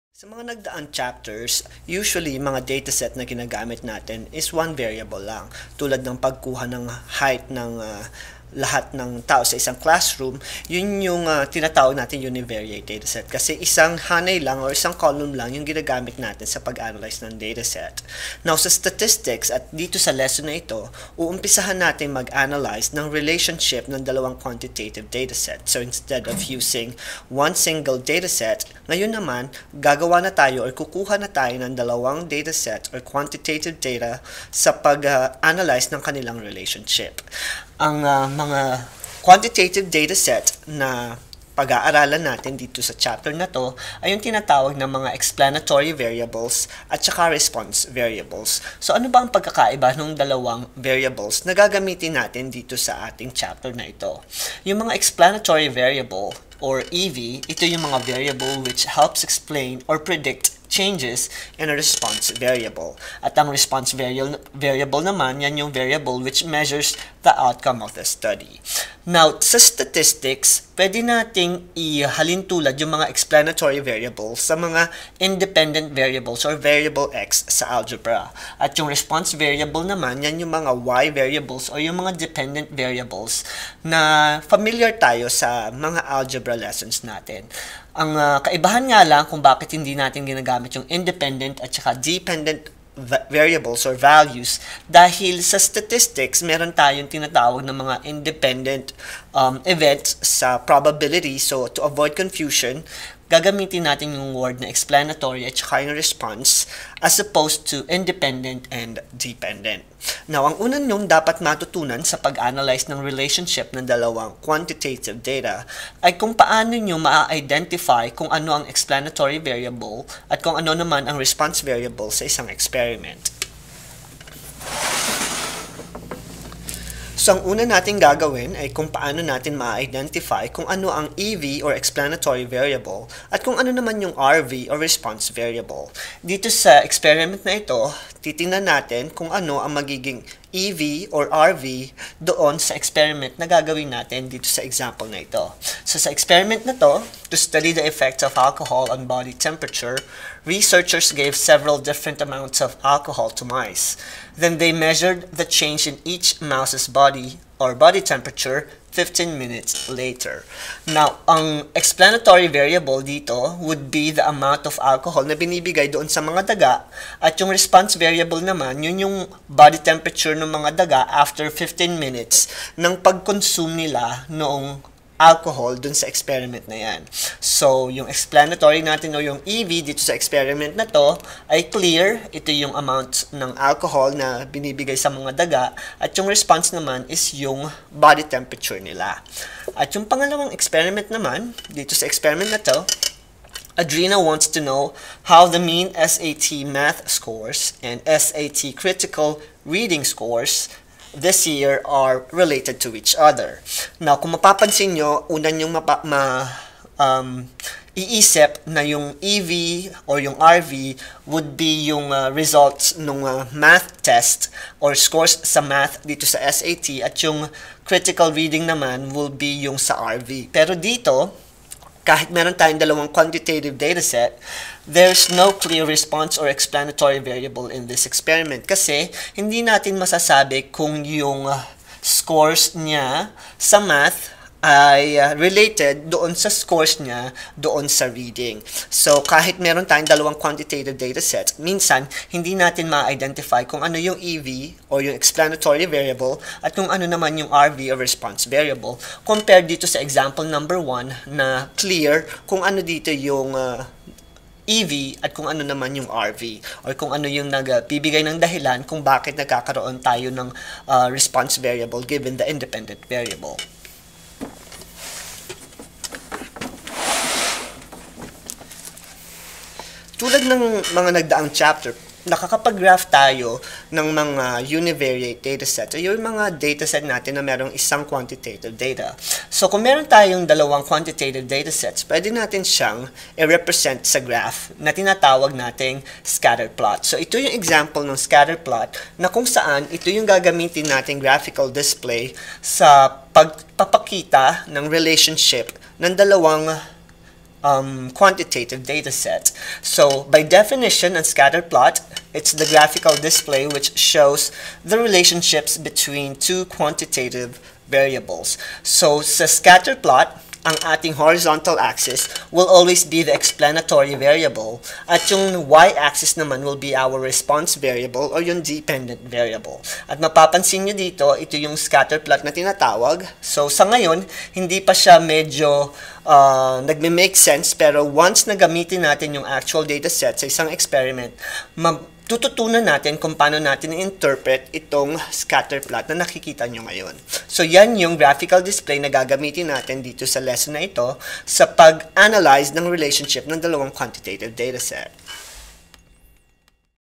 STATISTICS - Explanatory vs Response Variables in Filipino
Ang statistics lesson na ito ay nagpapakita ng pagkakaiba ng explanatory (independent) at response (dependent) variables.  Sa pag analyze ng linear relationship ng 2 quantitative data, kailangan na maidentify muna ang mga variables para makapagperform ng linear regression.